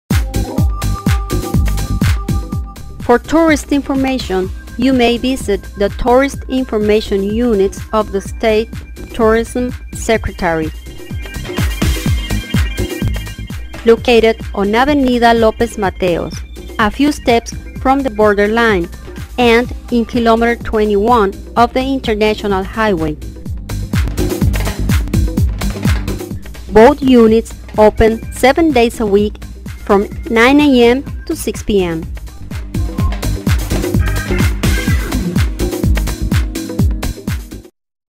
Tourism Offices in Nogales Sonora Mexico
Look for the "Tourist Information Unit of the State Tourism Secretary" in Downtown Nogales or if you are traveling on Mexico's International Highway 15 you can find an office in Km 21 complex